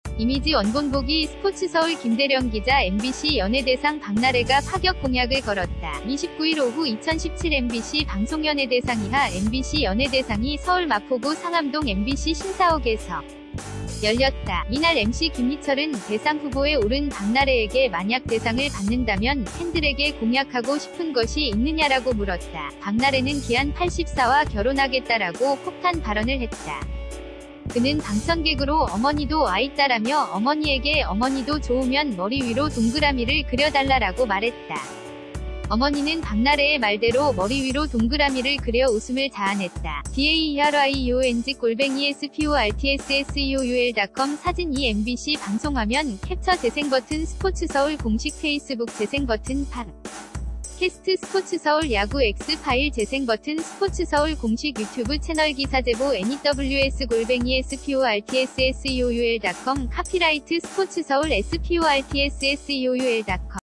[mbc 연예대상] 박나래 "대상 받으면 기안84와 결혼" 파격 공약- News Now
이미지 원본보기 스포츠서울 김대령기자 MBC 연예대상 박나래가 파격 공약을 걸었다.29일 오후 2017 MBC 방송연예대상이하 MBC 연예대상이 서울 마포구 상암동 MBC 신사옥에서 열렸다.이날 MC 김희철은 대상 후보에 오른 박나래에게 만약 대상을 받는다면 팬들에게 공약하고 싶은 것이 있느냐라고 물었다.박나래는 기안84와 결혼하겠다라고 폭탄 발언을 했다.